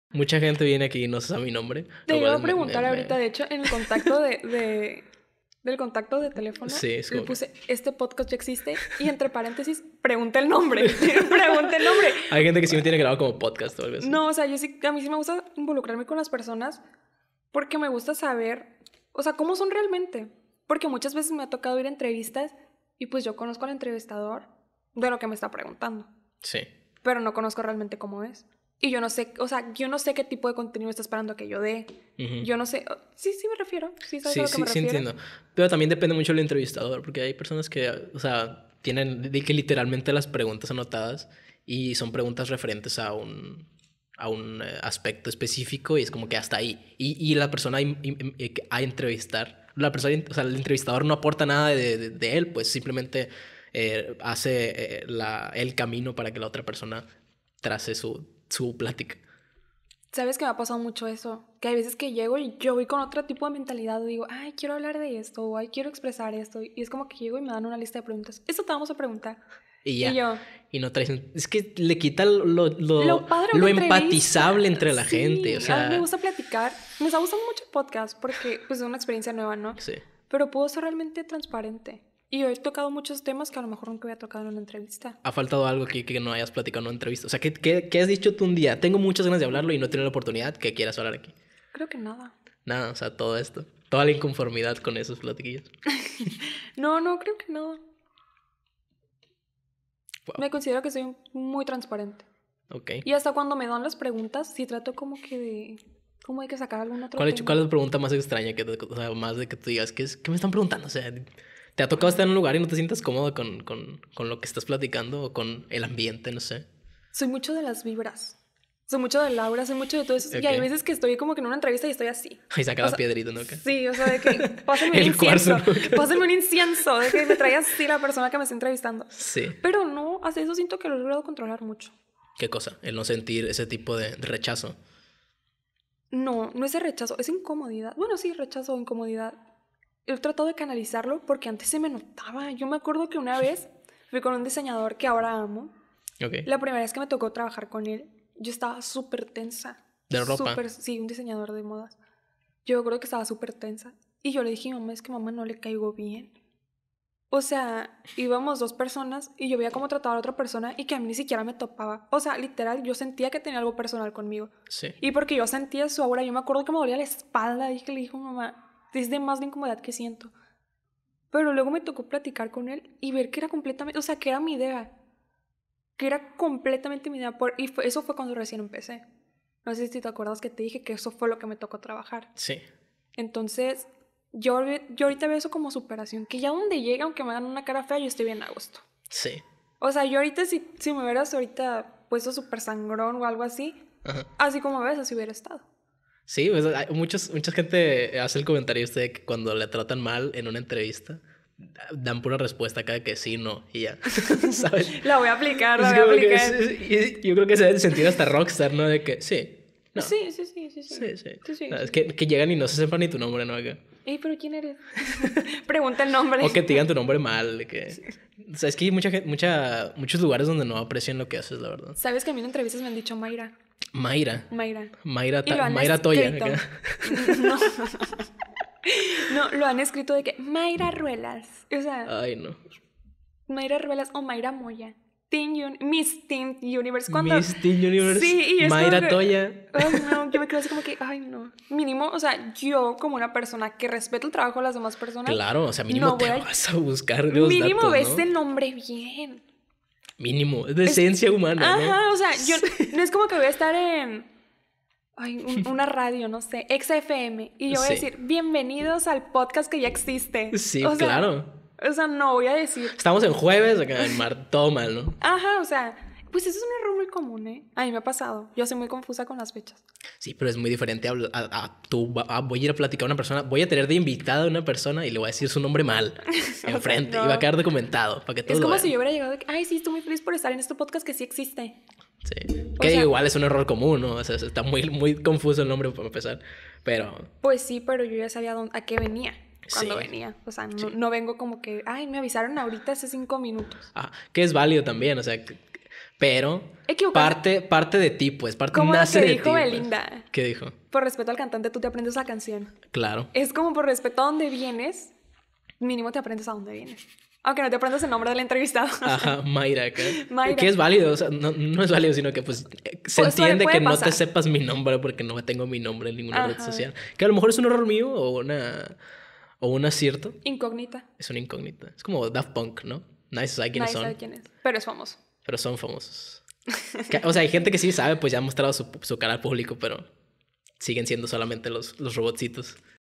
MALOS ENTREVISTADORES CON TAYRA MOYA II ESTE PODCAST YA EXISTE (O NO) II
Mis peores experiencias con gente de los medios de comunicación convencional.